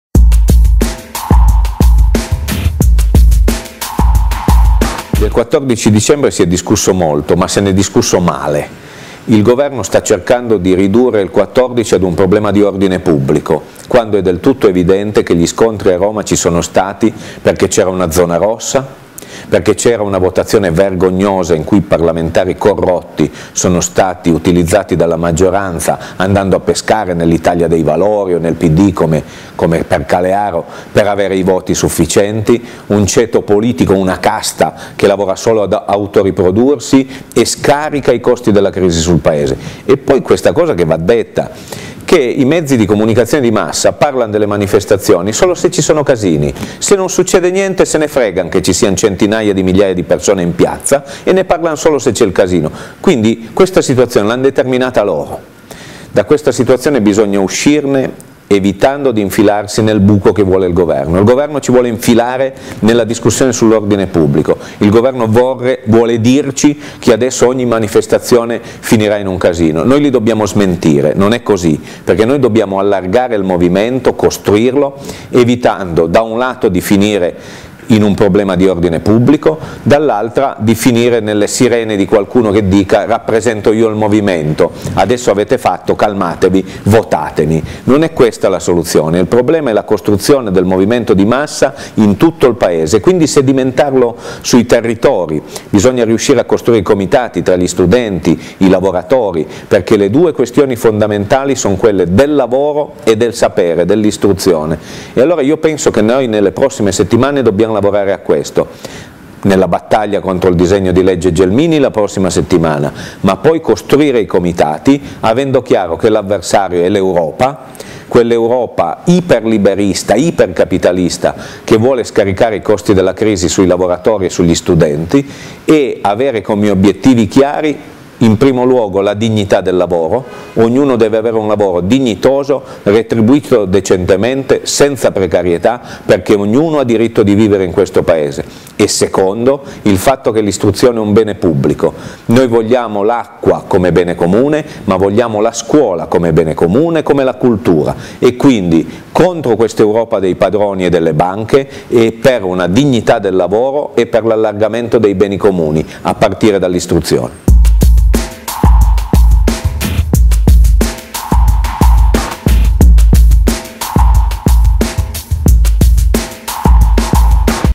Paolo FERRERO: DOPO IL 14 DICEMBRE NON CADIAMO NELLA "TRAPPOLA" - 17.12.10
INVISIBILE-IMPERDIBILE è un progetto Web TV ideato, realizzato e prodotto da Federica Pezzoli's Channel per il Partito della Rifondazione Comunista